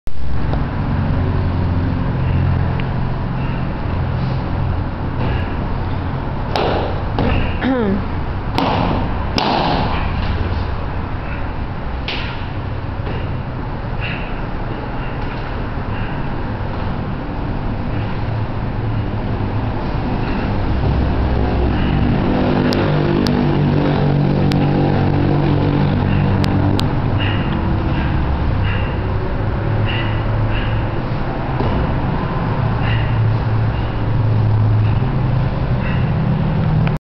kung kuen
Forma del programa para cinta negra primer grado de las escuelas dragón de fuego Casas en Guadalajara Jalisco México.